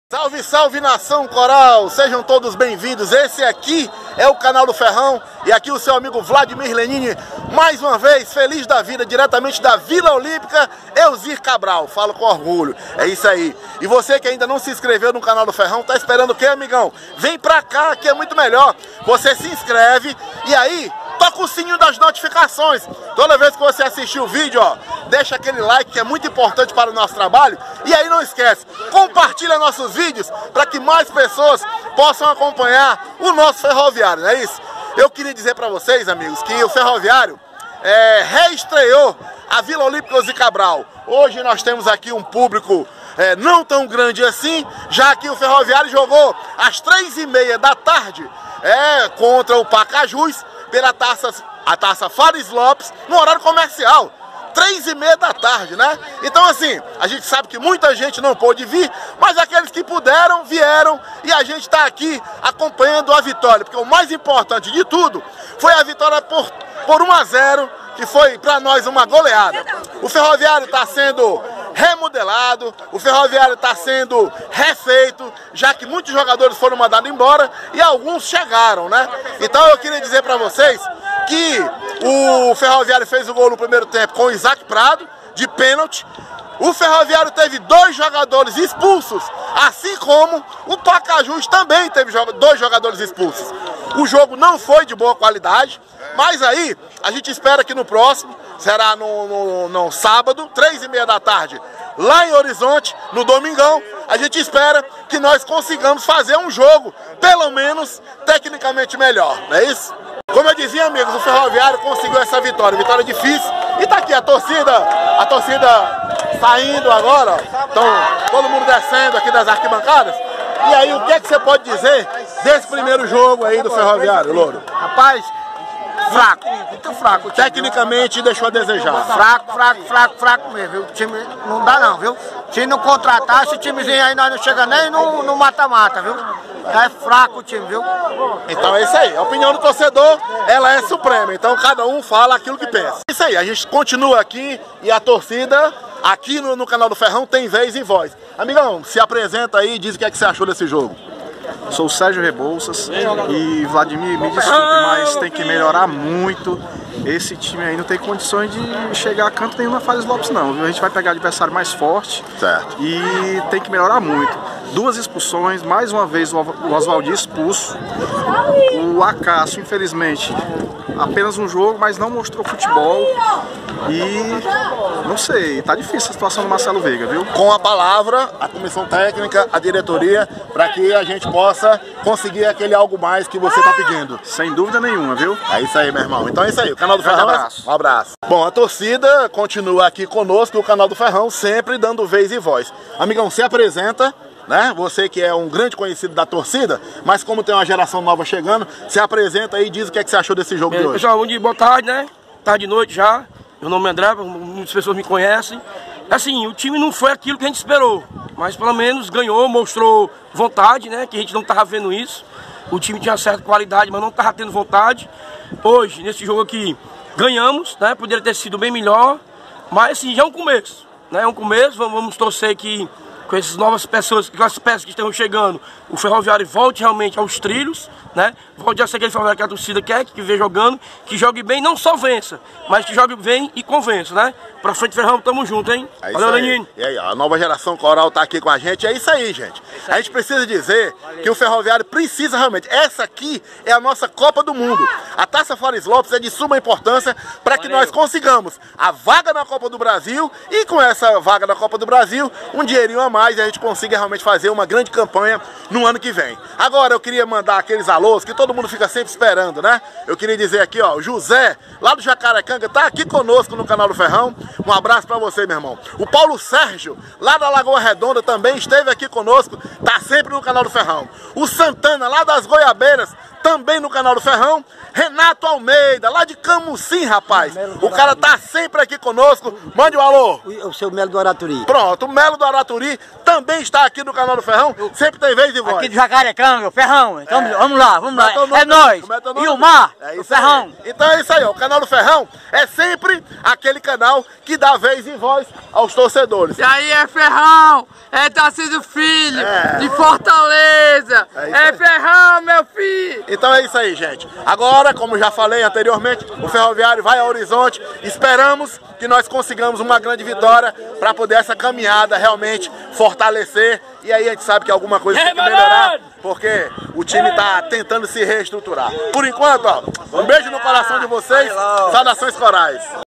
REESTREIA NA BARRA
Ferroviário vence o pacajus por 1x0 na Barra
ferrao vence pacajus